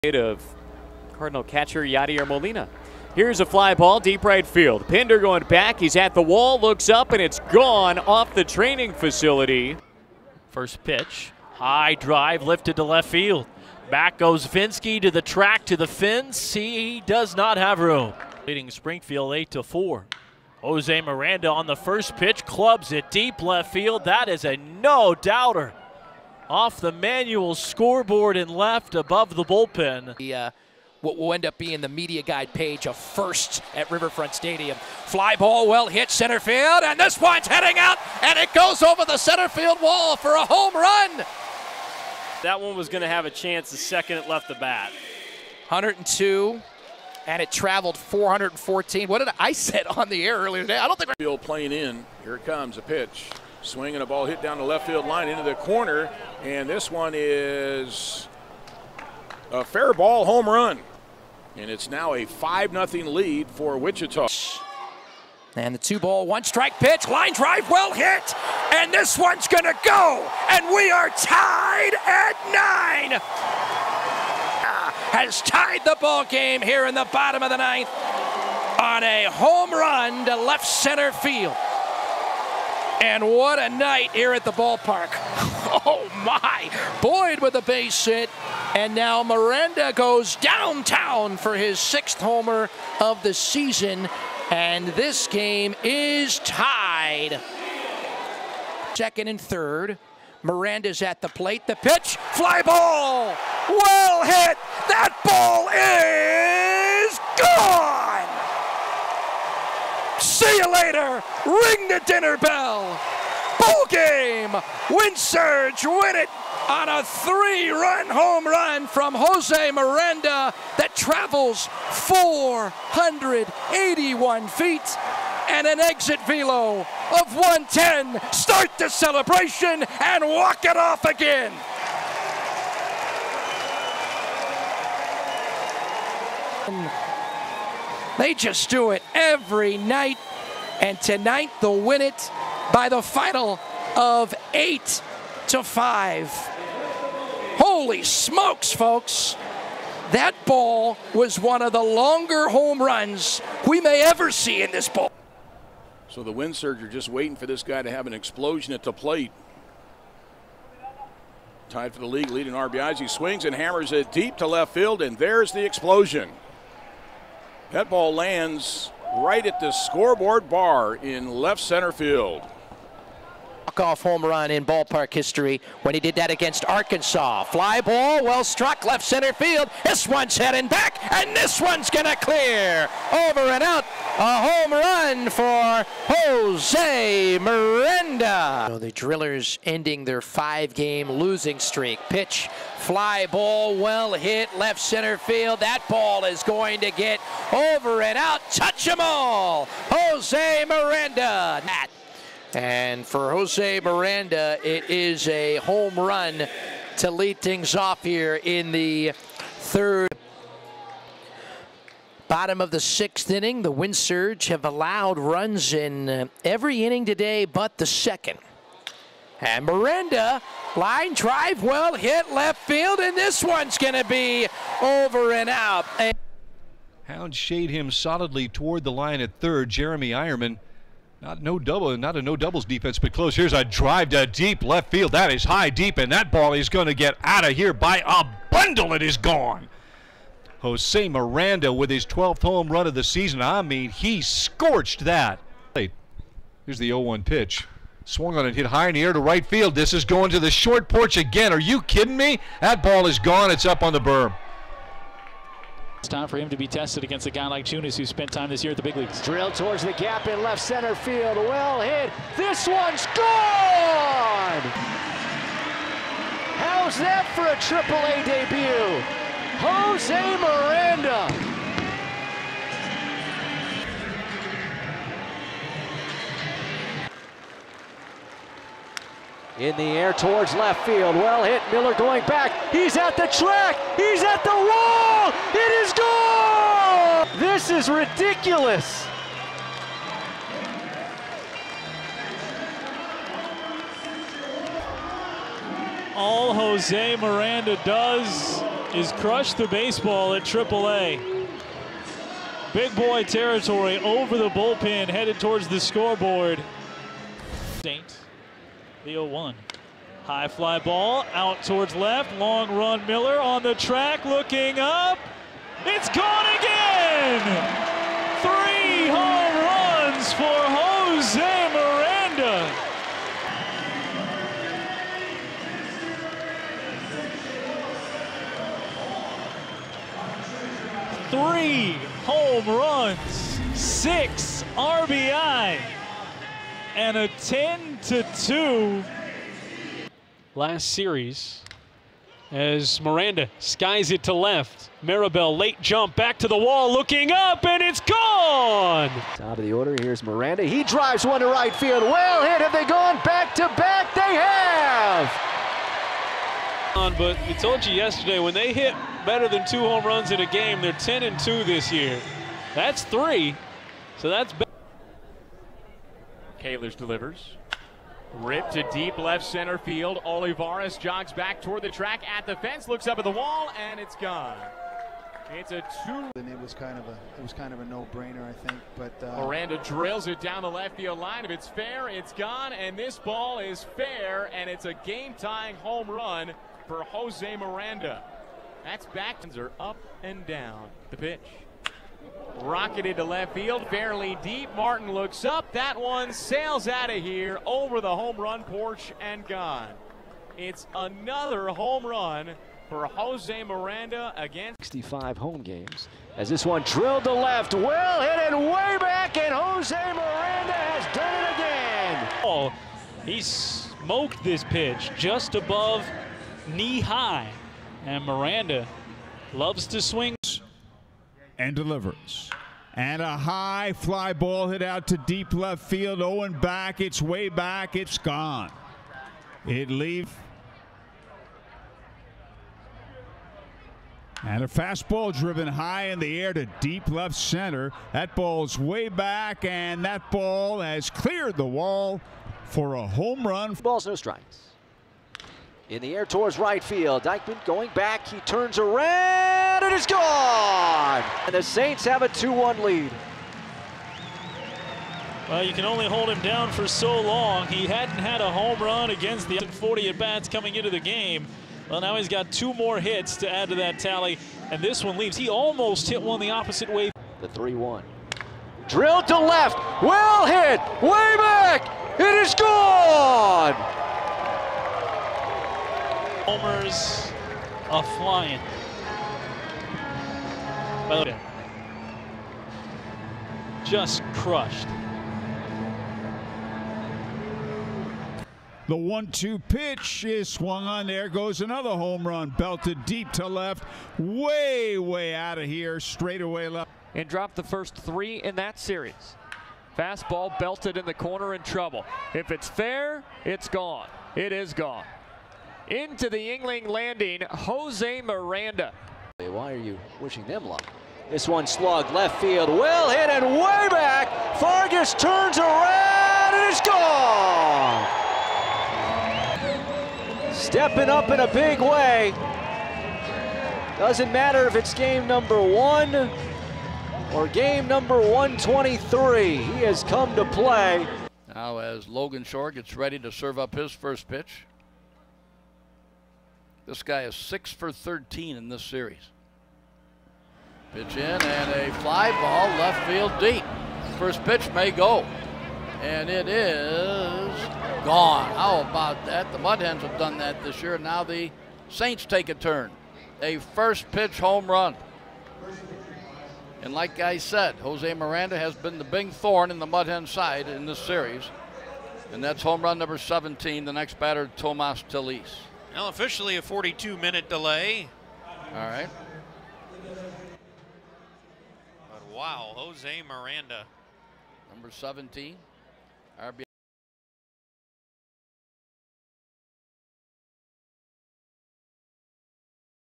Jose Miranda's 30 Home Runs | Minnesota Twins Prospect Highlights
Minnesota Twins prospect Jose Miranda hit 30 home runs in 2021. Here are highlights of all 30 of them.

*Channel formerly known as MN Twins Talk by Tom*